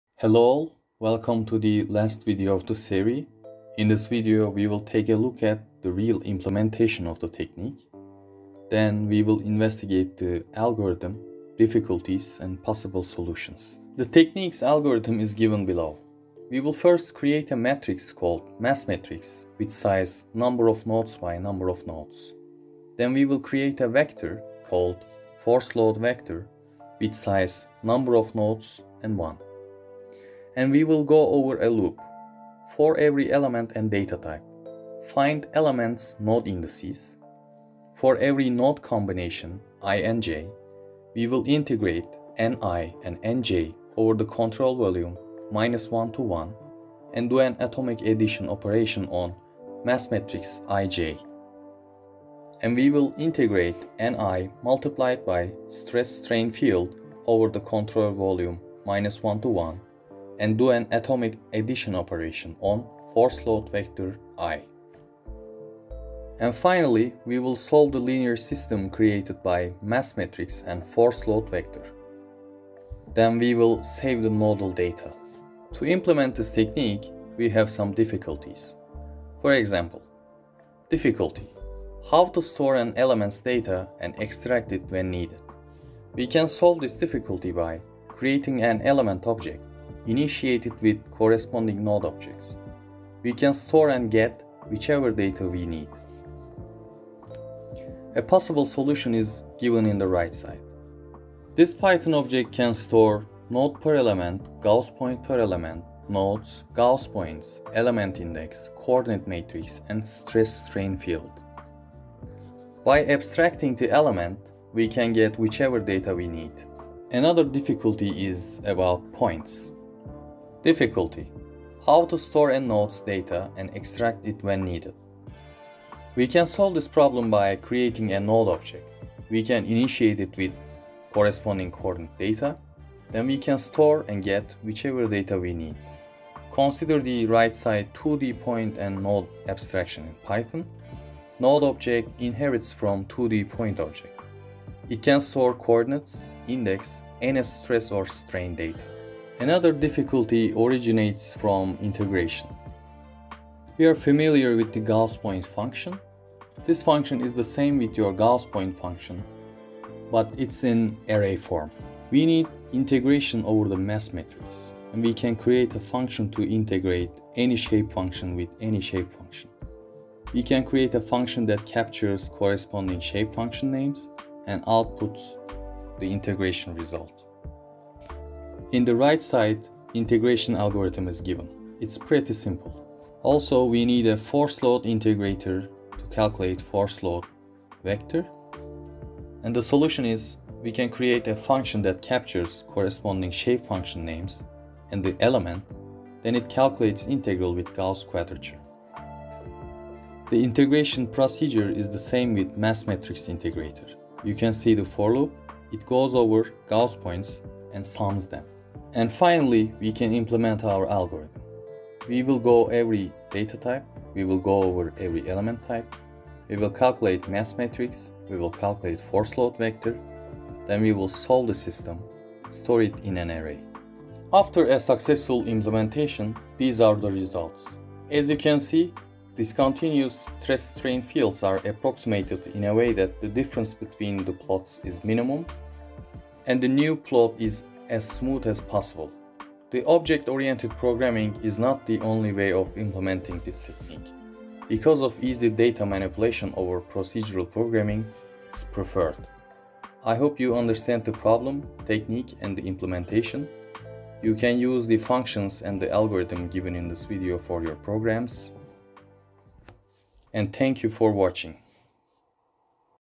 FEM - Stress-Strain Projection 4 - Implementation
Finite Element Method (FEM)

This is our global stress-strain projection video by Emre Erçelik providing basic implementation of the technique.